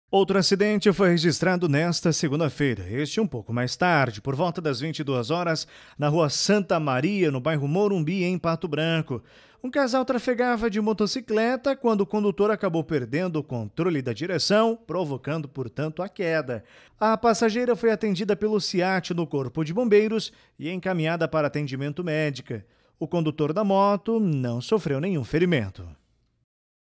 Acidente de motocicleta deixa um ferido em Pato Branco
Acidente: Durante a noite desta segunda-feira (09) uma queda de motocicleta acabou deixando uma pessoa ferida.